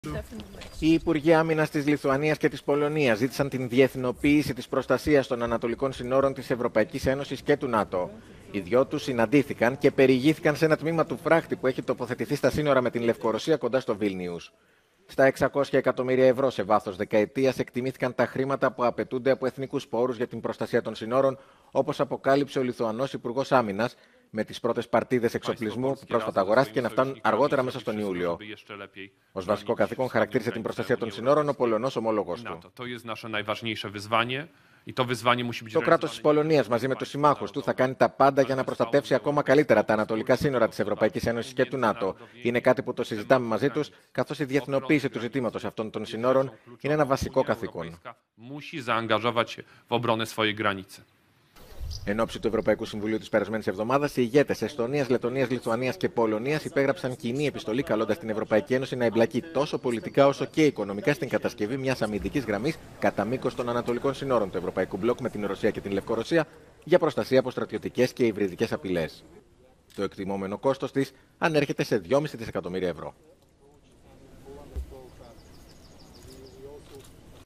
Ενίσχυση των ανατολικών συνόρων της ΕΕ ζητούν Πολωνία και Λιθουανία
Για αποτροπή πιθανών υδριδικών επιθέσεων από Ρωσία και Λευκορωσία